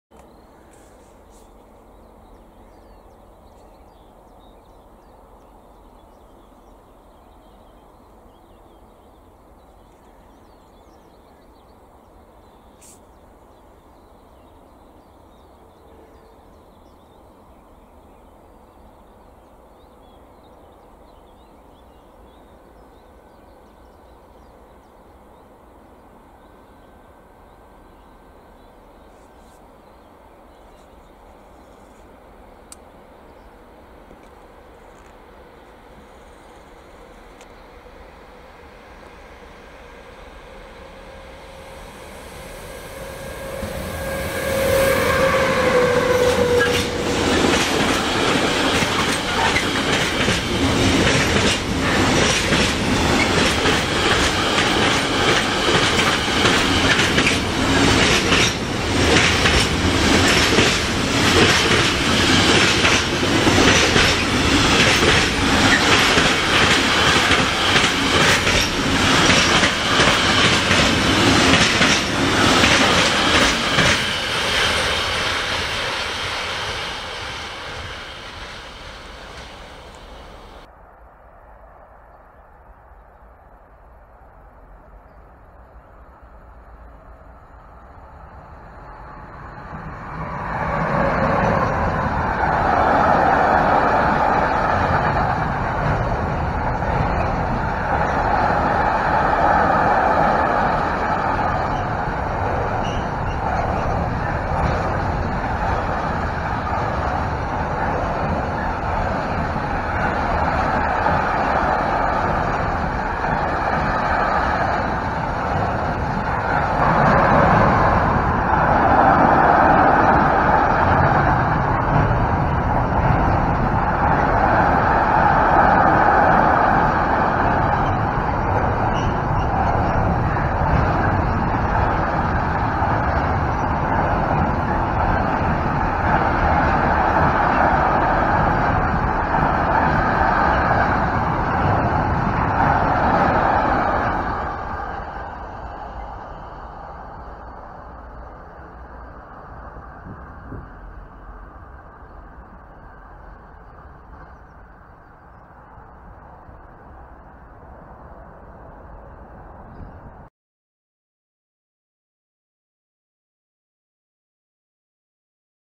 Containerzug am frühen Morgen
Kaum habe ich alles aufgebaut fährt eine 393 von METRANS mit ihrem Containerzug in Richtung Kreuz Wustermark an mir vorbei.